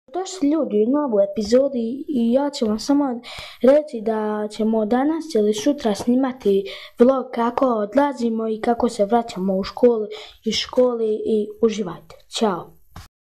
Obavjest!
Zdravo ljudi danas smo snimili obavjest da cemo uskoro snimiti vlog